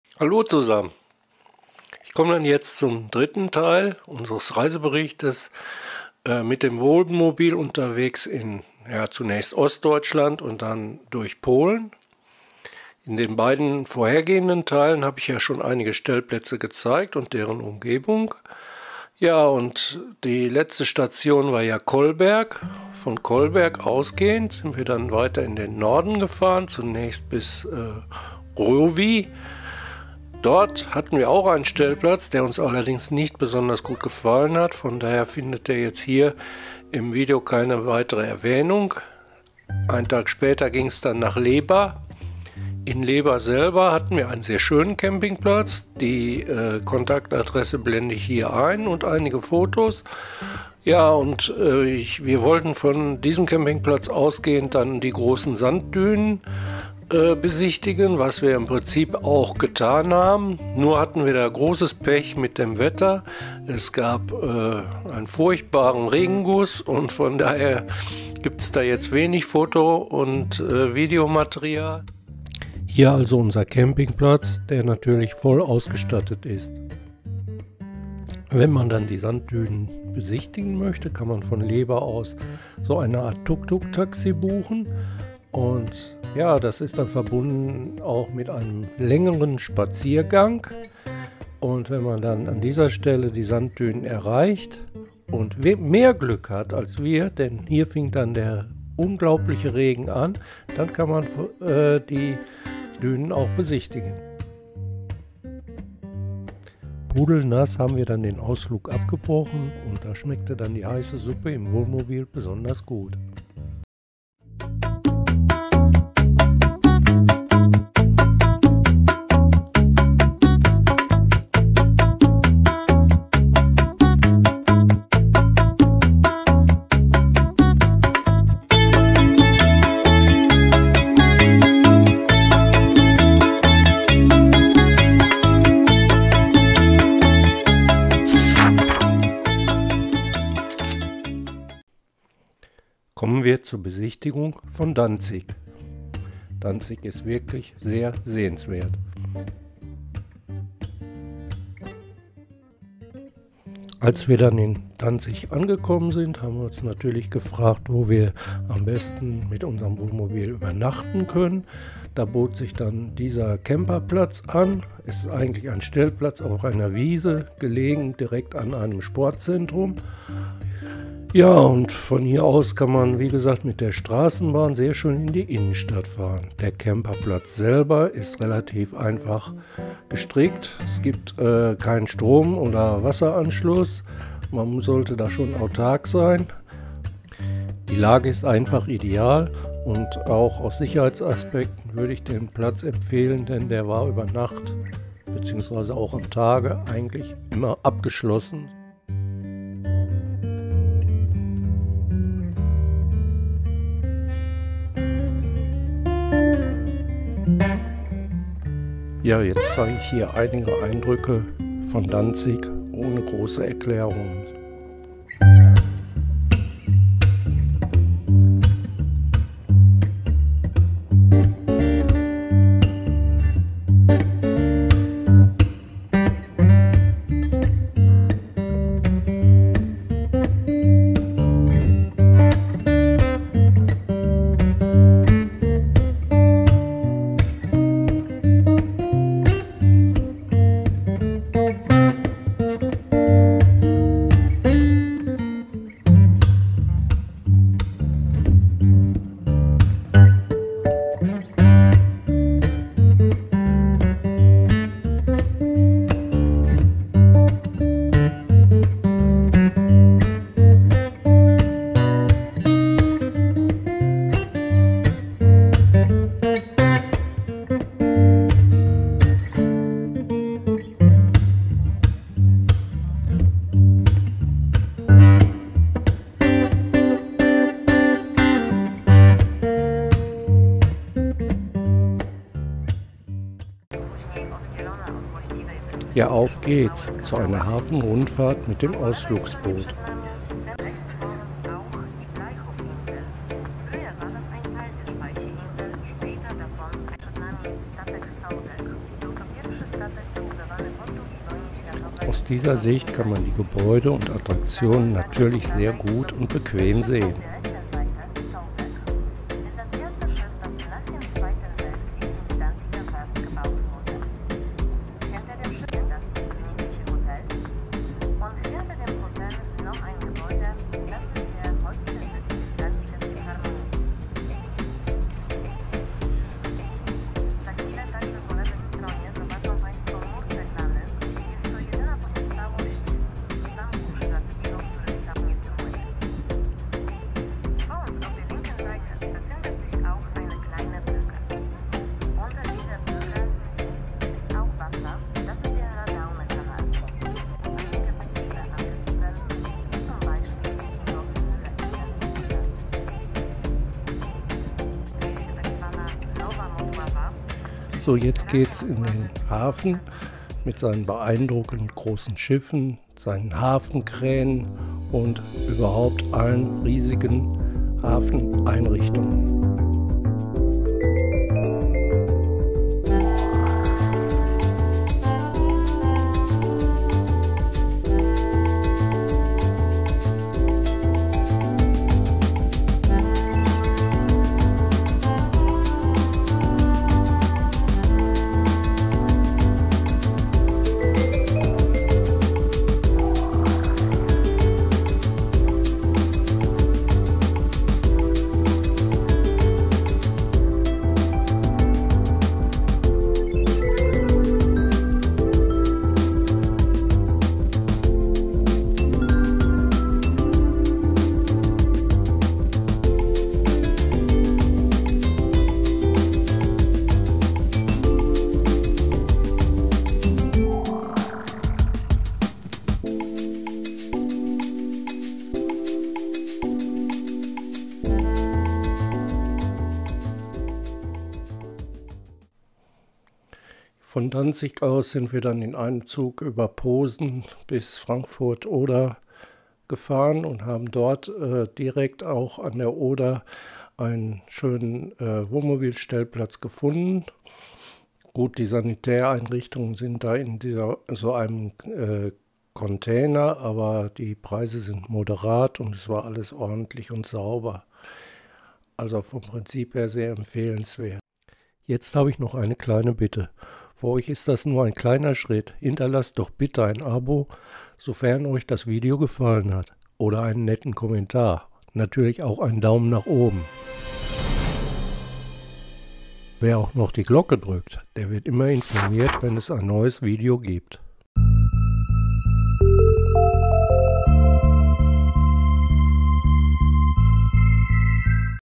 Wohnmobilreise Polen Teil 3 Stell- Campingplätze in Leba, Danzig, Frankfurt/Oder
Im August 2021 waren wir auf Wohnmobil Tour in Ostdeutschland und Polen unterwegs. Dabei sind viele Foto und Video Aufnahmen entstanden.
Dieses Video zeigt 3 Wohnmobil Stellplätze in Leba, Danzig und Frankfurt / Oder. Ausgehend von diesen Stellplätzen haben wir einige touristische Highlights besucht.